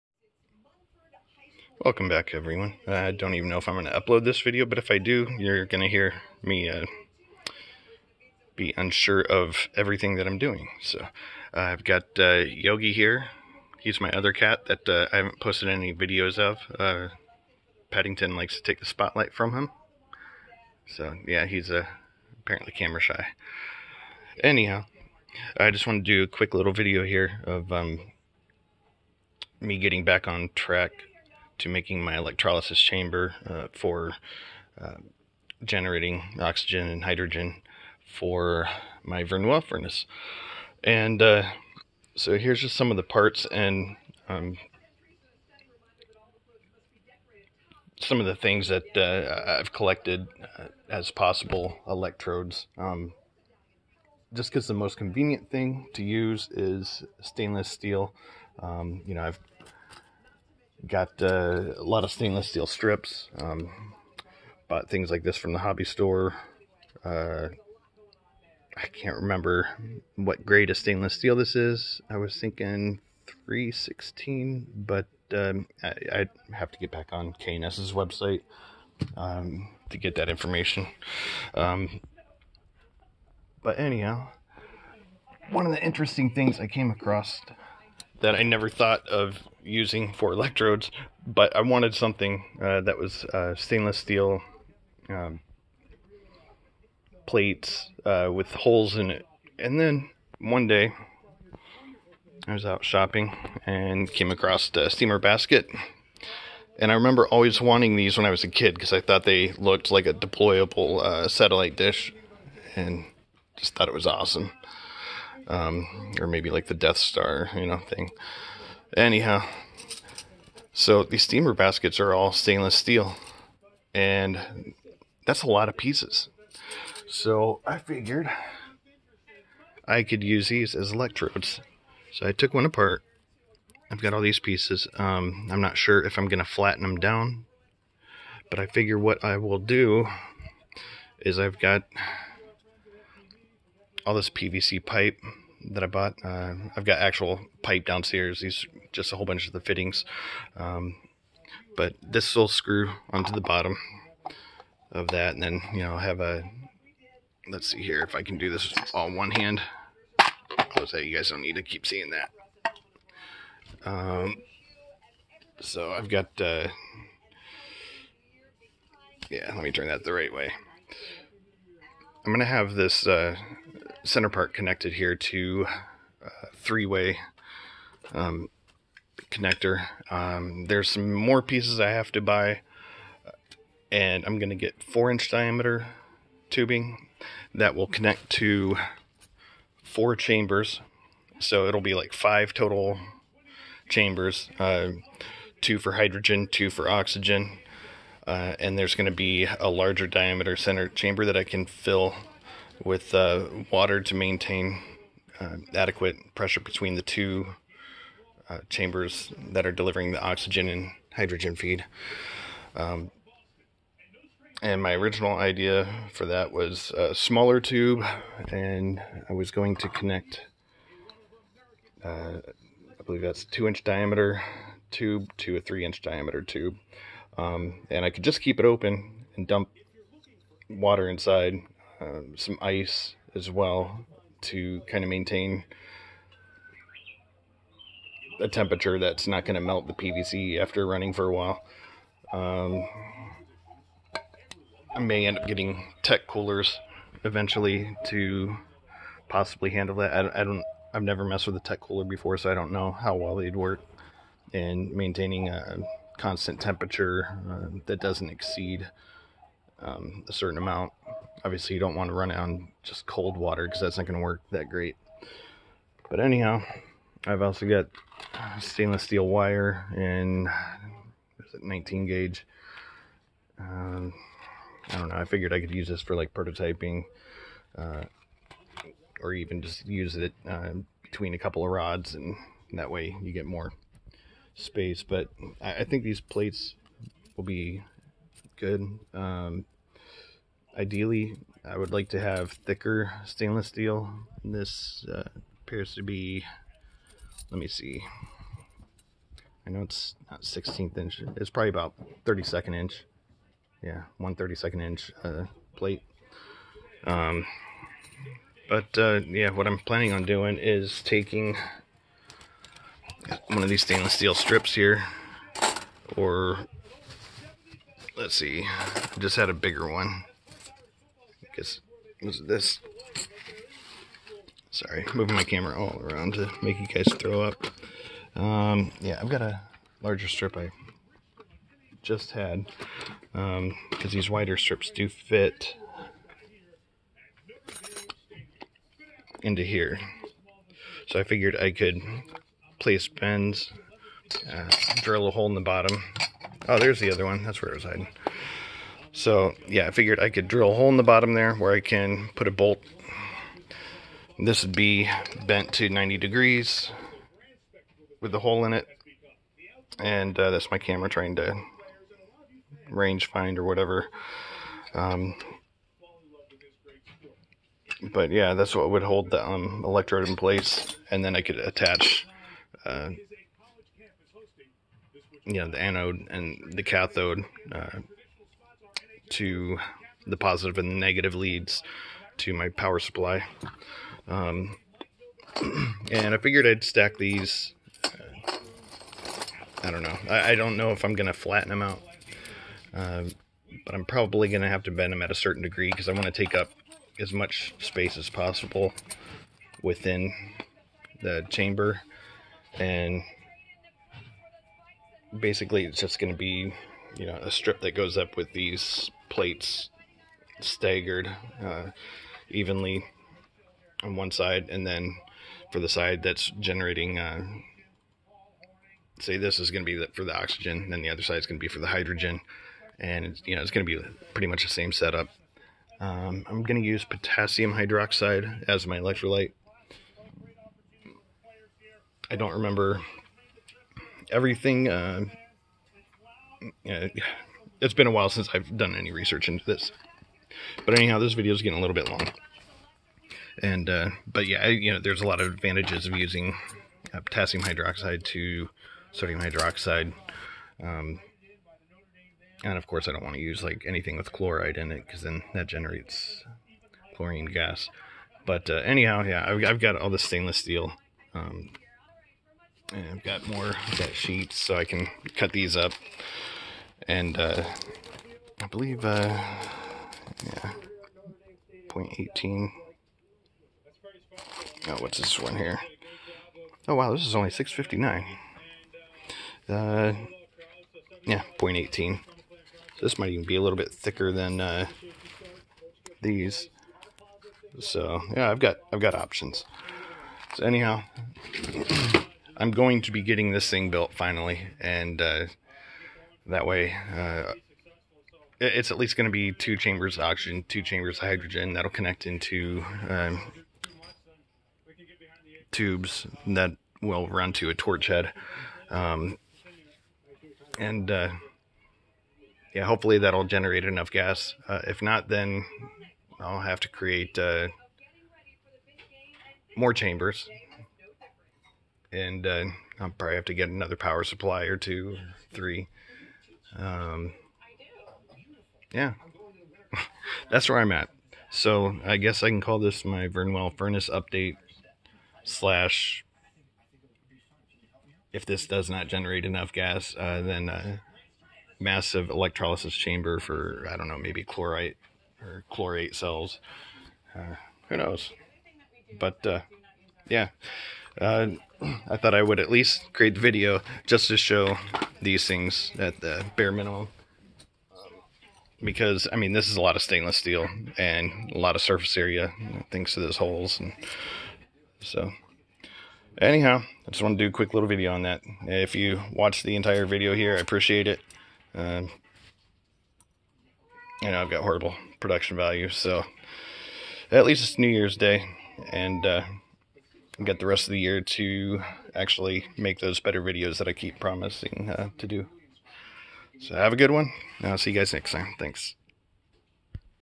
Hydrogen + Oxygen Generator: Build Concepts
I am finally beginning construction of my electrolysis apparatus to generate oxygen and hydrogen fuel for the Verneuil furnace.

Deciding on the final design has been quite a hold-up for me over the past year for many reasons. The main reason for having this project on the back burner for so long is I've been enjoying learning chemistry and performing as many experiments as I possibly can. I've also been designing this entire project from the ground up and producing enough gas to operate the furnace has been a serious challenge. The torch head I designed will require a large volume of hydrogen and oxygen to function properly. Compromising on torch flame size has been rather difficult, as I am investing so much time and money into building a furnace capable of growing synthetic ruby, sapphire and rutile via flame fusion / the Verneuil process.

I am not keen on the idea of HHO torch setups for several reasons, which is why I am so specific on stating oxygen + hydrogen generation. My decision is my personal preference. I don't think any less of those that build and use HHO torches safely and effextively. With that being said, the HHO torches I have seen use hypodermic/syringe needles as the torch head and the flames are usually rather small. I have seen setups producing larger flames, however the setup I am building will be generating an abundance of gas which I am uncomfortable handling if mixed together at the fuel generation source, and ran through the entire system to the torch head.

I had initially thought 10 LPM could be sufficient, but I believe this to be insufficient for producing quality boules of ruby, and most especially sapphire. I'd rather not waste time and resources building something which produces an unsatisfactory end product. I'm not stating the target volume of fuel generation as of yet as I am unsure how much of a compromise of torch flame size I can accommodate at this time.